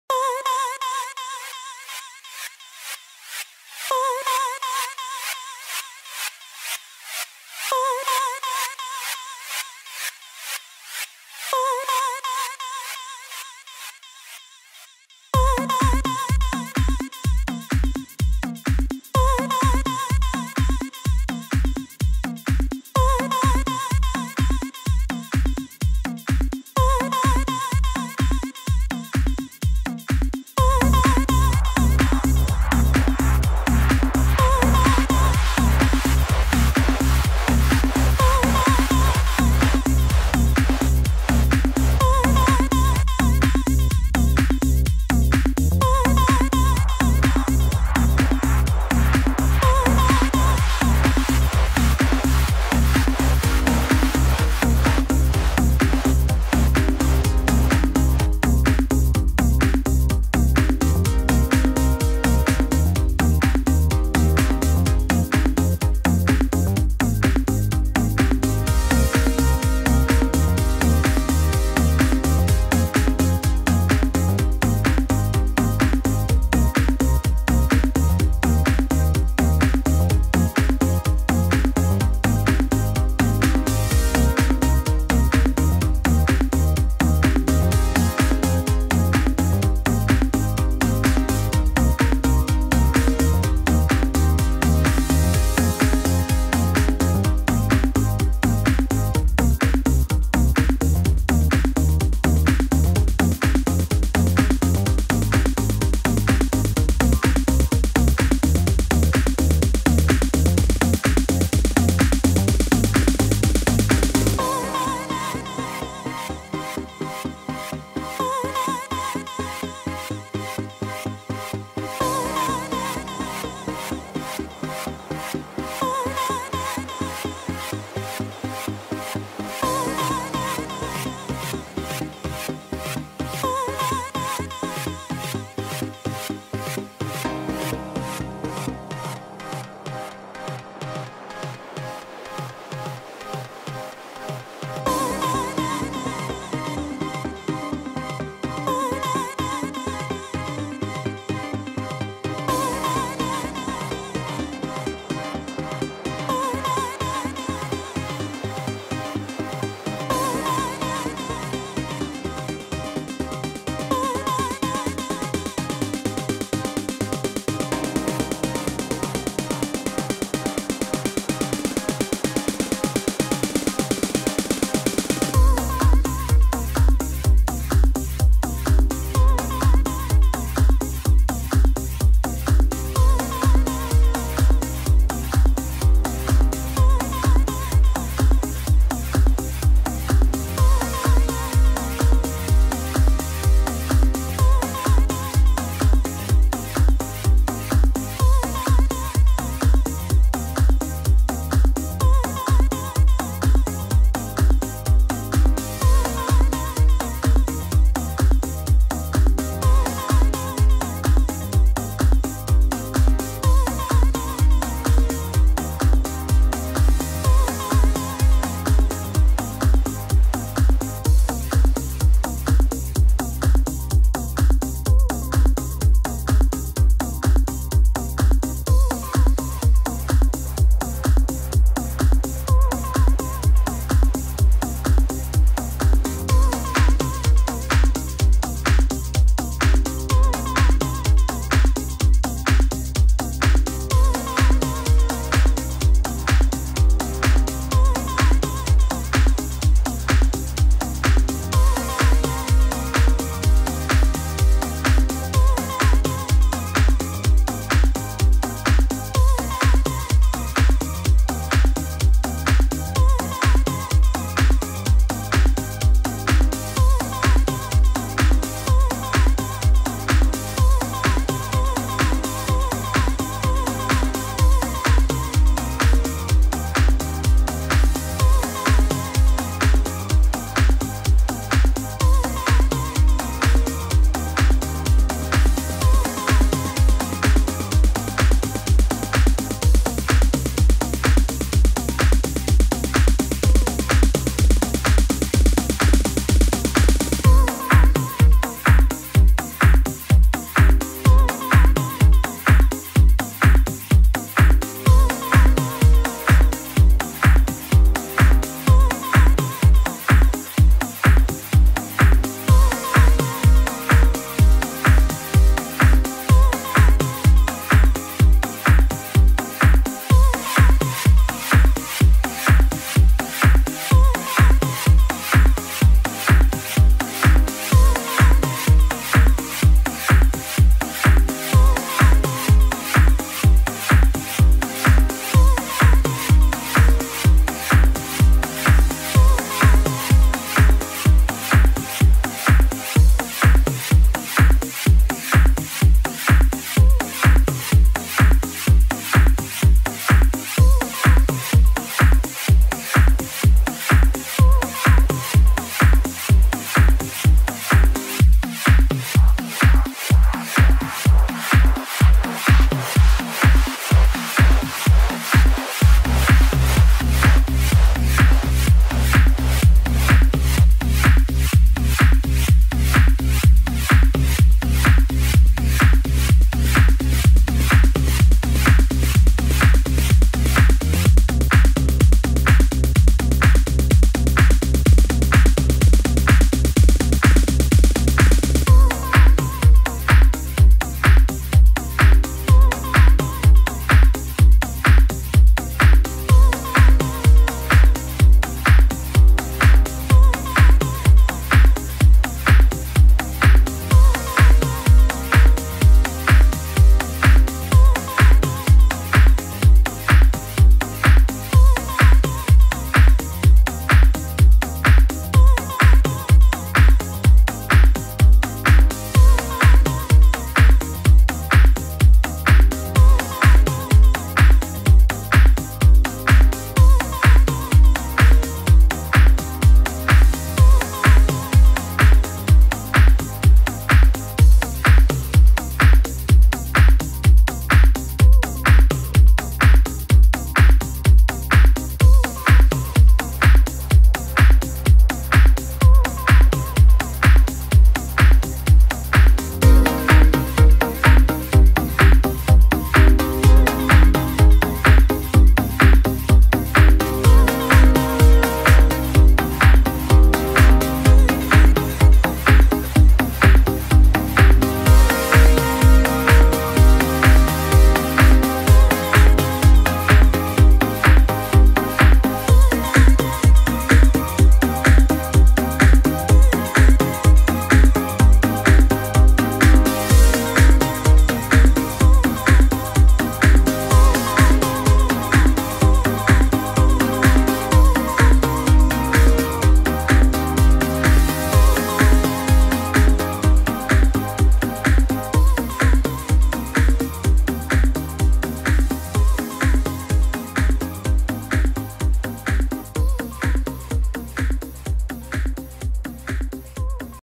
High Speed Rail
Swedish House/ trance Mix, Simple Music video celebrating Floridas first High Speed train